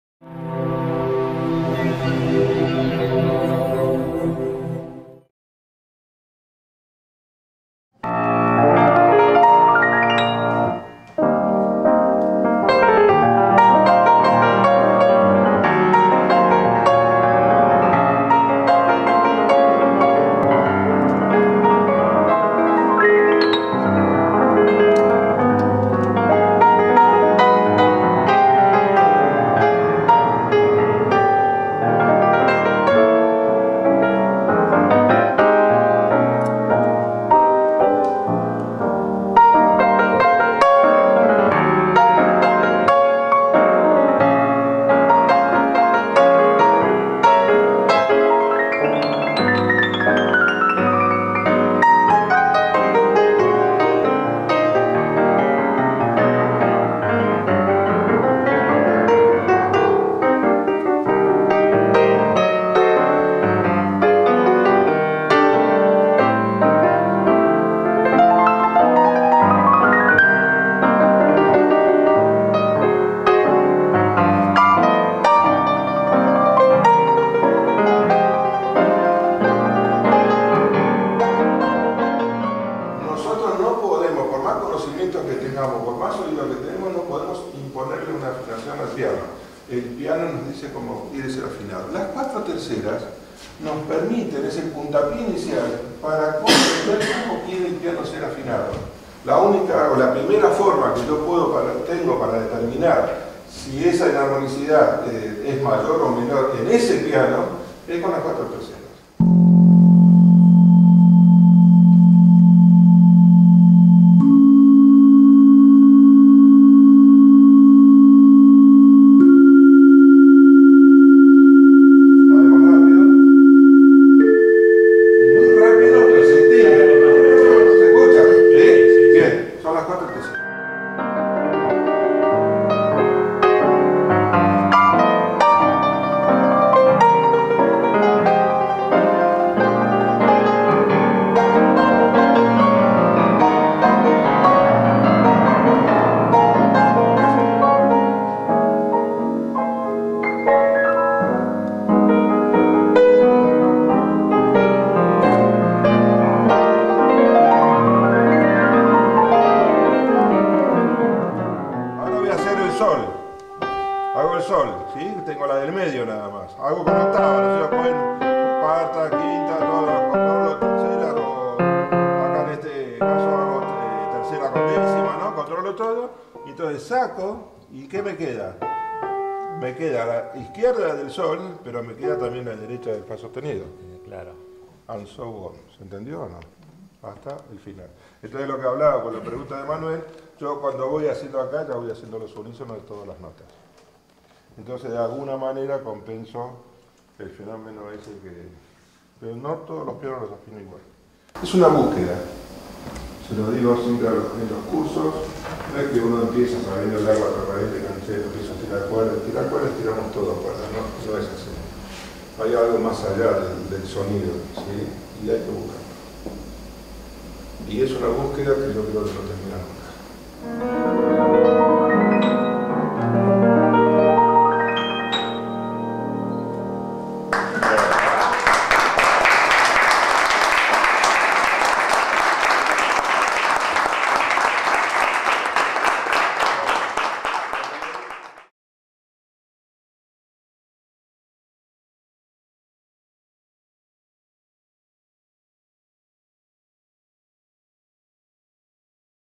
Clase Magistral Agosto 2014
El pasado 16 de agosto de 2014,se realizó la Primer Clase Magistral de Técnicas de Afinación de Pianos, a cargo del Técnico afinador Juan Alberto Schultis, quien actualmente es el único Técnico Afinador de Pianos en Argentina Certificado por la PTG como Registered Piano Technician (RPT).

El objetivo fue brindar una clase teórico/práctica para compartir información, novedades y experiencia,  en pos de colaborar en la mejora de las técnicas de afinación, el rendimiento y la sonoridad de cada instrumento. Agradecemos a todos los alumnos, colegas y público en general que nos acompañó en ese día tan importante para nosotros.
Les mostramos un breve video del resumen de la jornada.